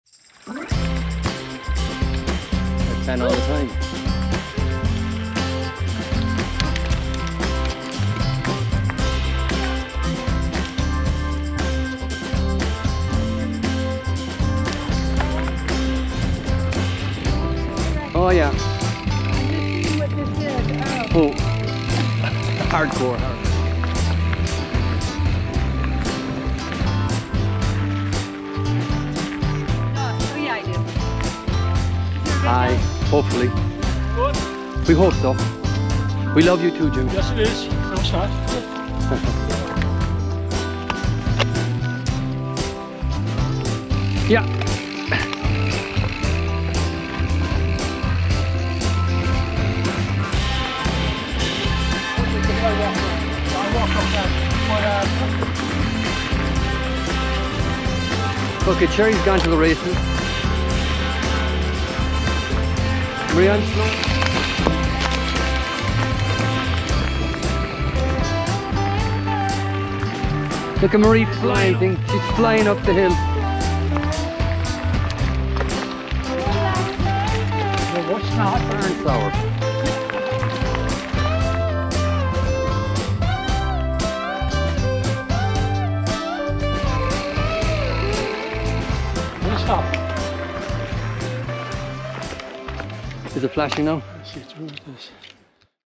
Arnold Ride
A quick video of our Arnold Rim Trail ride